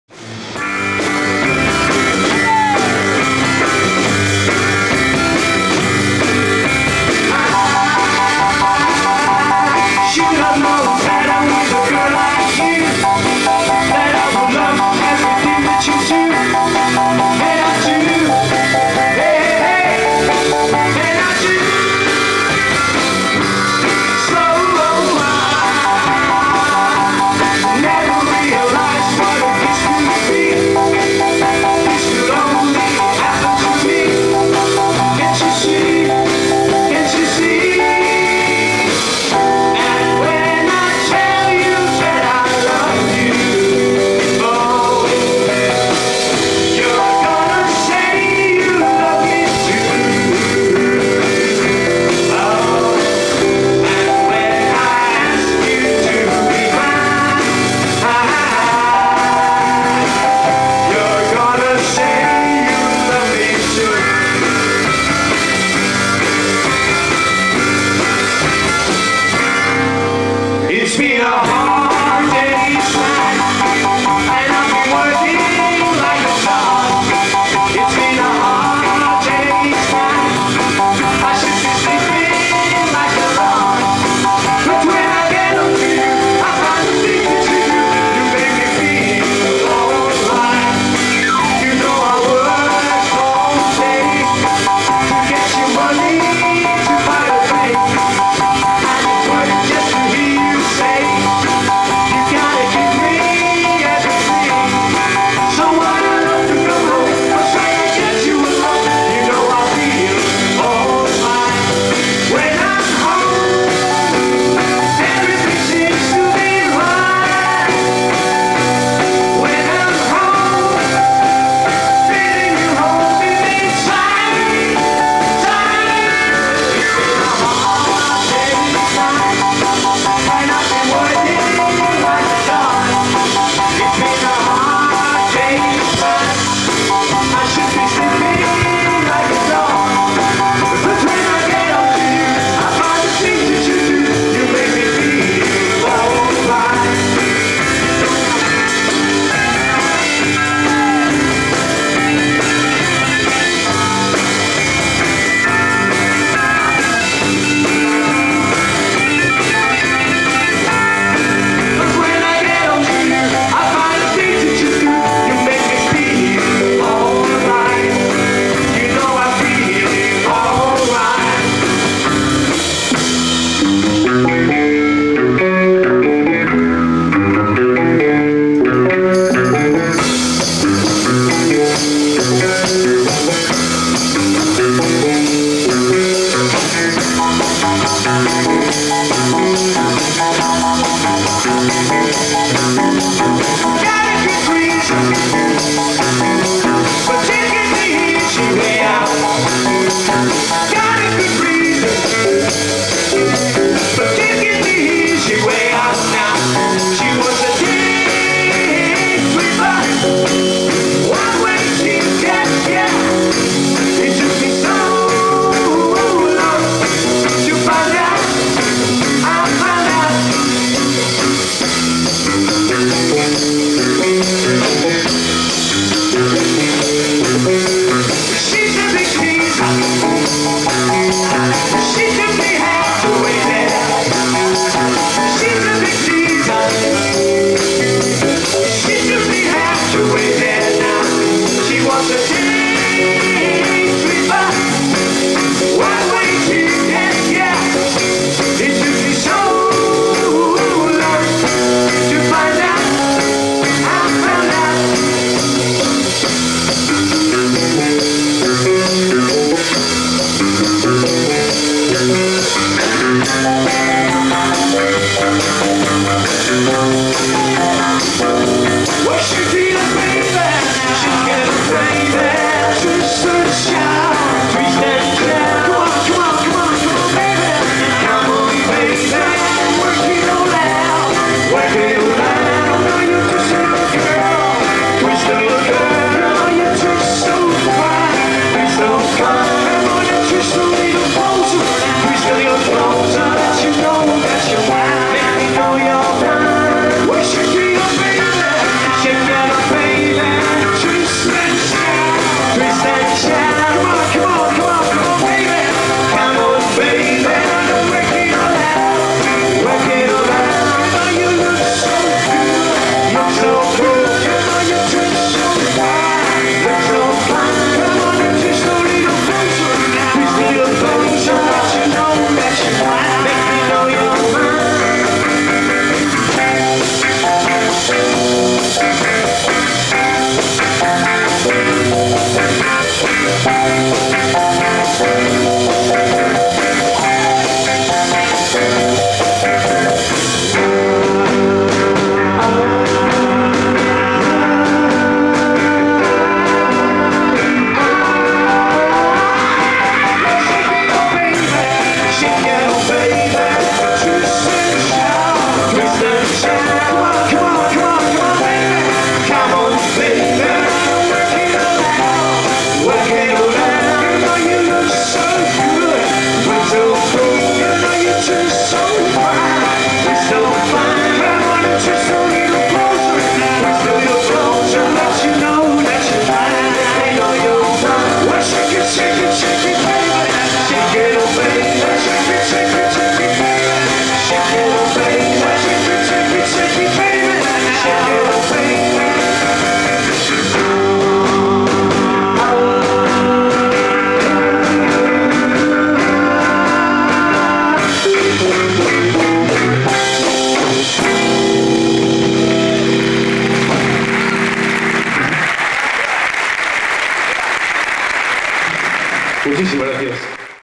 Ricky's Revival.mp4
Actuació dels Ricky's als jardins del Retiro el juny de 1987, després de 19 anys sense tocar. Les imatges estan enregistrades amb càmeres domèstiques de l'època, per tant no espereu qualitat. I del so encara menys; ja que és procedent d'una de les 3 càmeres que es van utilitzar i està saturat.